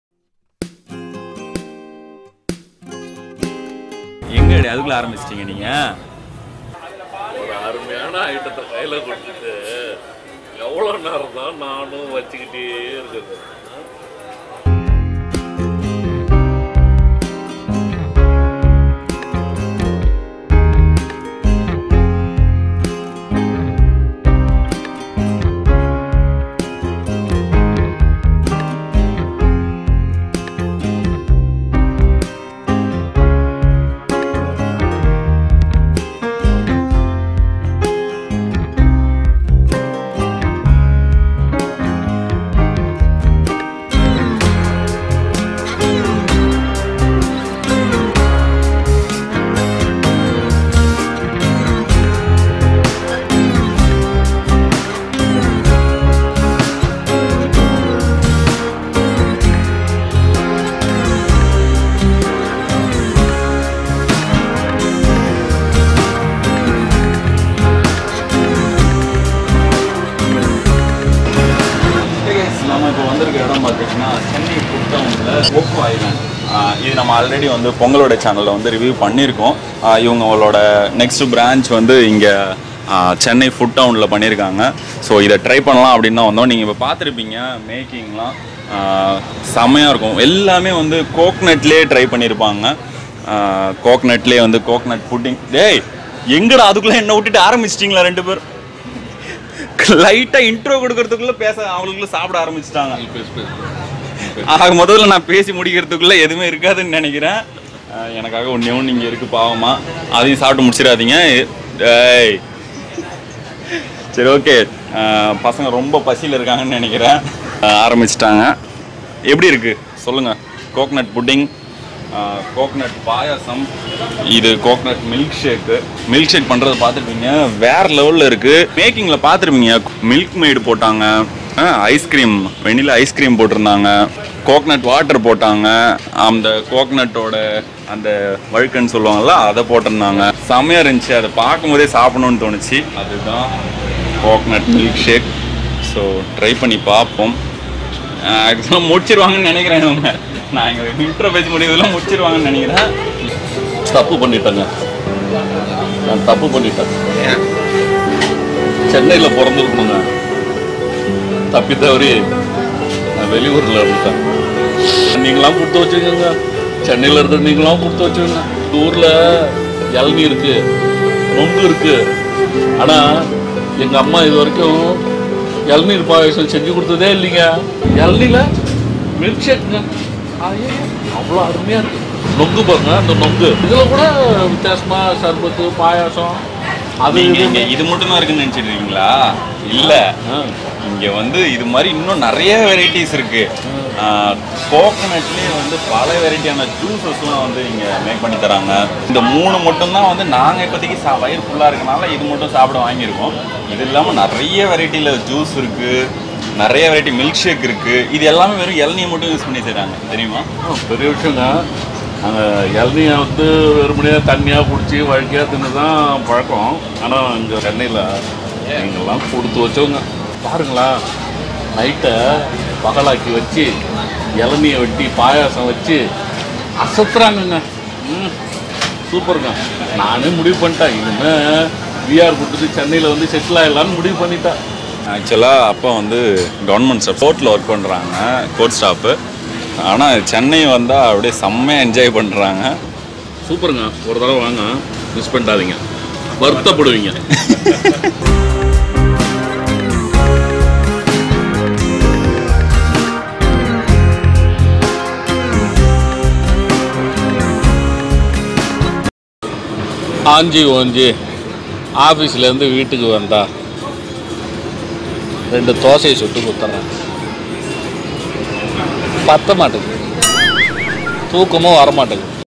Tamil Family Tasting Coconut Dishes Ep-4 | VJ Gowtham Vlogs
We have tasted coconut dishes in coco island, with my family.

Location: Coco Island, Chennai Food Town, Thoraipakkam, Chennai, Tamil Nadu.